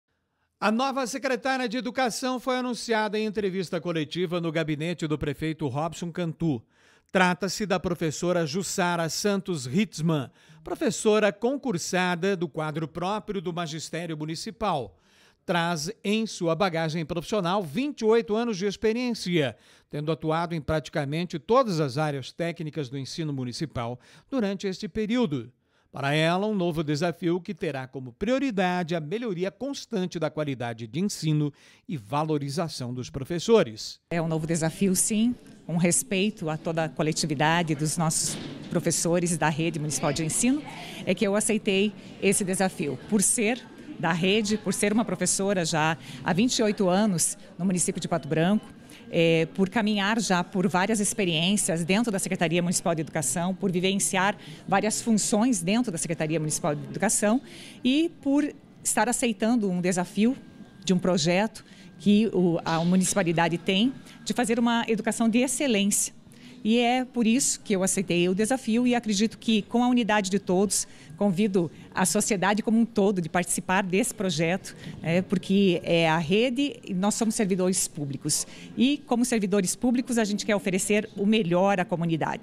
Toma posse a nova Secretária de Educação de Pato Branco
Política: Pato Branco conhece a nova Secretária de Educação, empossada na manhã desta quinta-feira (18).